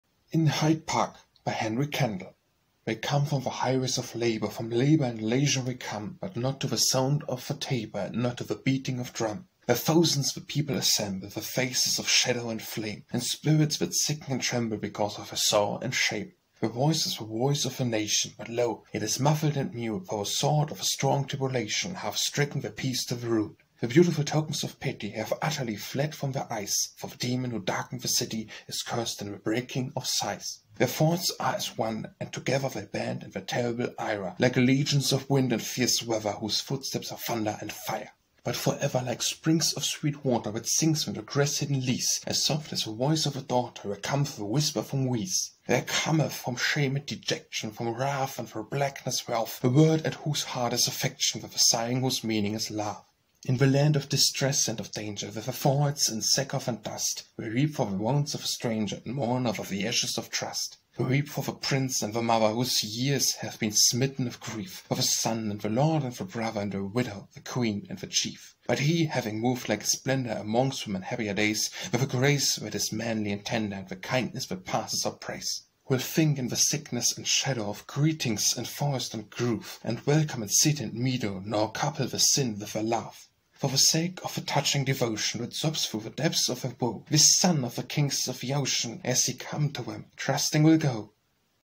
In Hyde Park - Henry Kendall
In Hyde Park
Henry Kendall

    They come from the highways of labour,
    From labour and leisure they come;
    But not to the sound of the tabor,
    And not to the beating of drum.

    By thousands the people assemble
    With faces of shadow and flame,
    And spirits that sicken and tremble
    Because of their sorrow and shame!

    Their voice is the voice of a nation;
    But lo, it is muffled and mute,
    For the sword of a strong tribulation
    Hath stricken their peace to the root.

    The beautiful tokens of pity
    Have utterly fled from their eyes,
    For the demon who darkened the city
    Is curst in the breaking of sighs.

    Their thoughts are as one; and together
    They band in their terrible ire,
    Like legions of wind in fierce weather
    Whose footsteps are thunder and fire.

    But for ever, like springs of sweet water
    That sings in the grass-hidden leas
    As soft as the voice of a daughter,
    There cometh a whisper from these.

    There cometh from shame and dejection,
    From wrath and the blackness thereof,
    A word at whose heart is affection
    With a sighing whose meaning is love.

    In the land of distress and of danger,
    With their foreheads in sackcloth and dust,
    They weep for the wounds of the Stranger
    And mourn o er the ashes of trust!

    They weep for the Prince, and the Mother
    Whose years have been smitten of grief
    For the son and the lord and the brother,
    And the widow, the queen and the chief!

    But he, having moved like a splendour
    Amongst them in happier days,
    With the grace that is manly and tender
    And the kindness that passes all praise,

    Will think, in the sickness and shadow,
    Of greetings in forest and grove,
    And welcome in city and meadow,
    Nor couple this sin with their love.

    For the sake of the touching devotion
    That sobs through the depths of their woe,
    This son of the kings of the ocean,
    As he came to them, trusting will go.